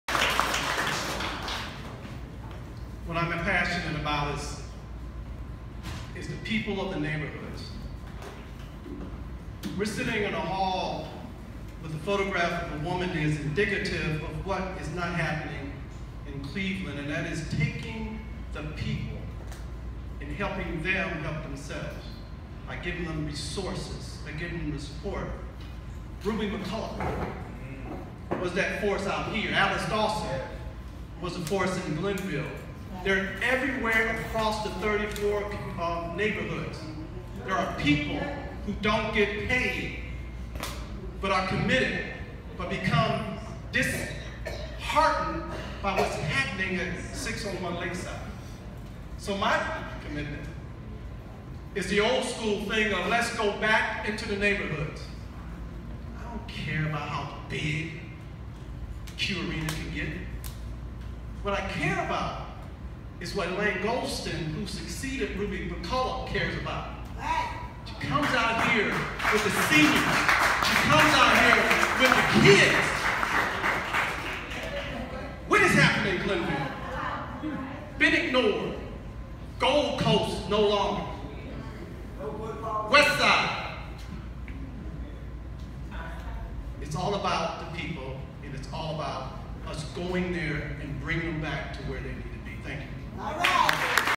Councilman Jeff Johnson speaks at CLE Mayoral Forum about empowering local citizens
Councilman Jeff Johnson speaking at CLE eastside Mayoral forum last night re empowering local citizens in their neighborhoods.